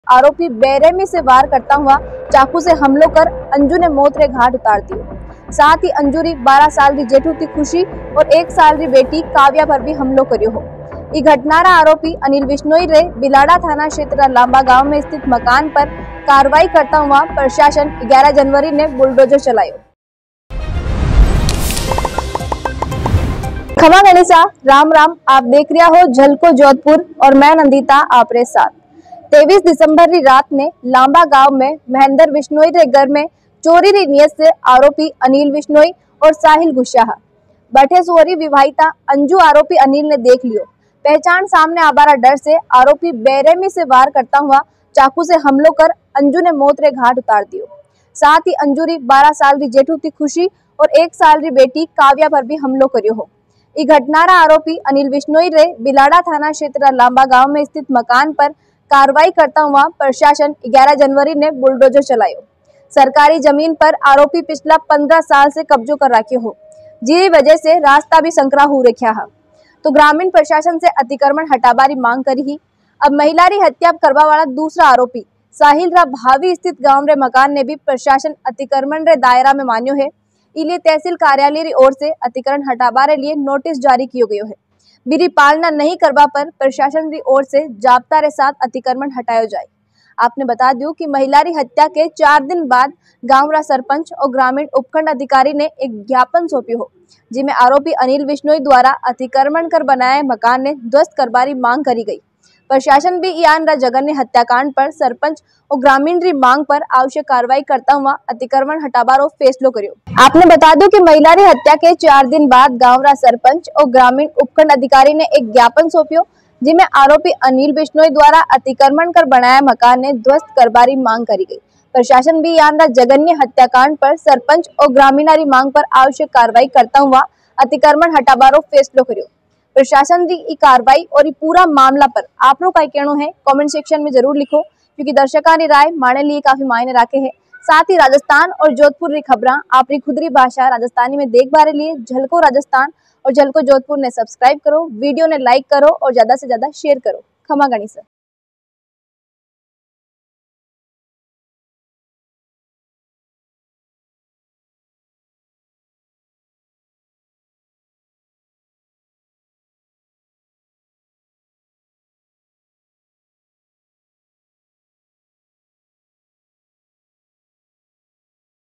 जोधपुर लाम्बा गांव हत्याकांड मामले में नया अपडेट देखें वीडियो ~Jhalko Jodhpur
Produced By: Jhalko Jodhpur
Edited By: Mukesh Bohara
Reported By : Nandita Ji
Cameraman : Lalit Ji